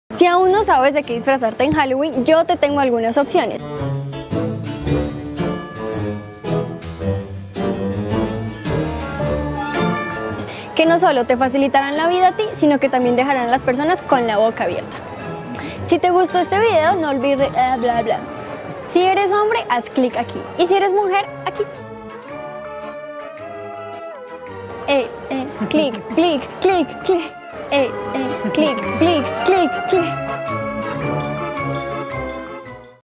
¿Cómo maquillarme para Halloween?
Disfruta de estos tutoriales para niño y niña y encuentra una forma sencilla y original de maquillarte para Halloween-